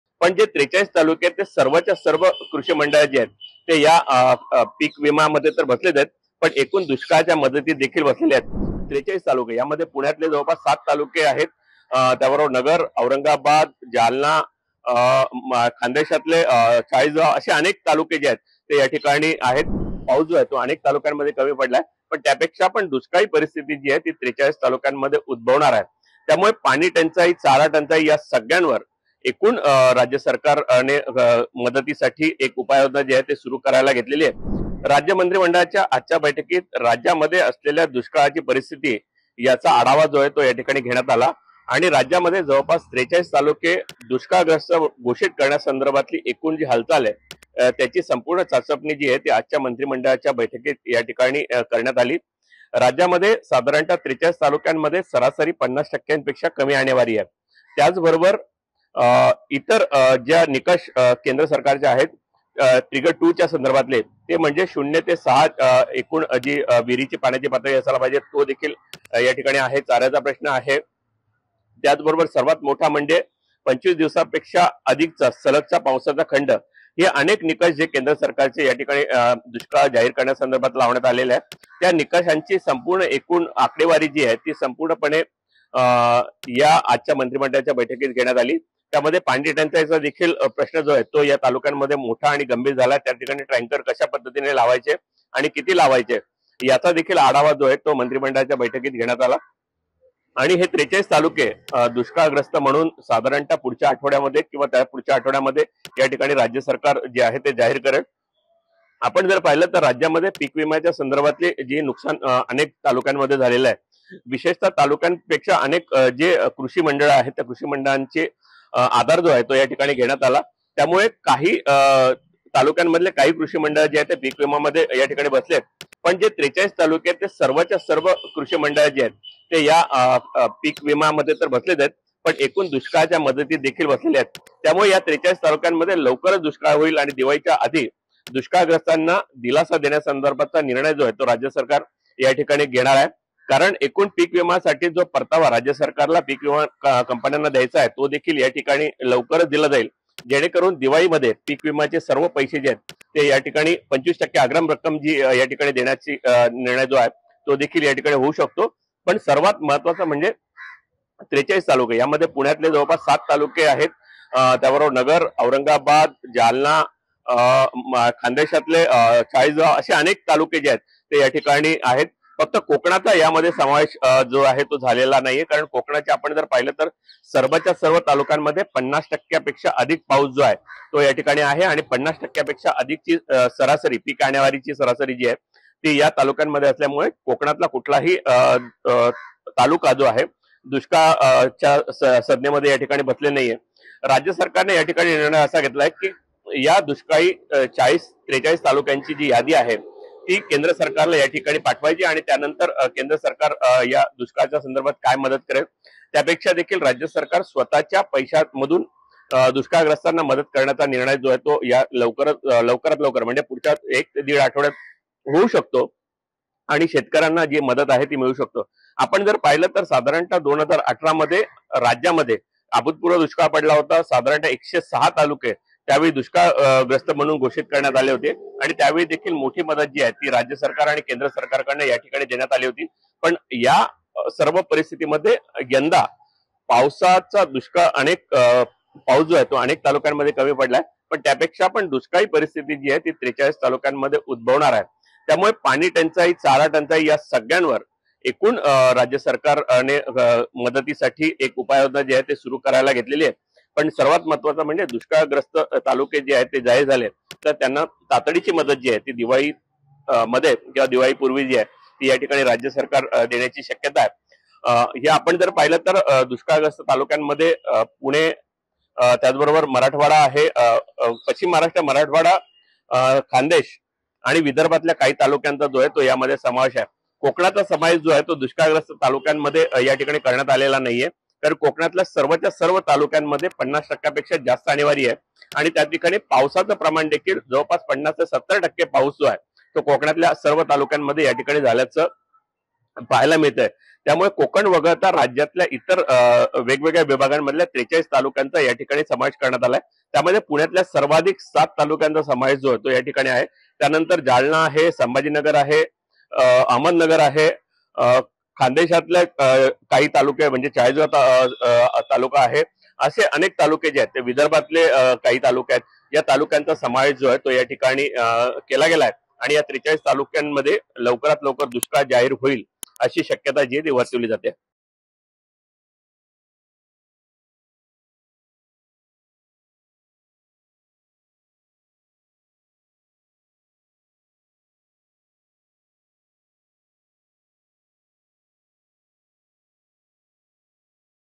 Drought Condition : राज्यातील दुष्काळाच्या सावटाखाली कोणत्या भागातील तालुके? | LetsUpp Marathi
राज्याच्या विविध भागातील 43 तालुक्यांमध्ये दुष्काळ जाहीर केला जाणार आहे. त्यासाठी मंत्रालयात विविध अंगांनी चर्चा झाली? दुष्काळ कधी जाहीर होणार? काय उपाययोजना आखल्या जाणार? यावर लेट्सअपचे विशेष प्रतिनिधी प्रफुल्ल साळुंखे यांनी विश्लेषण केलंय.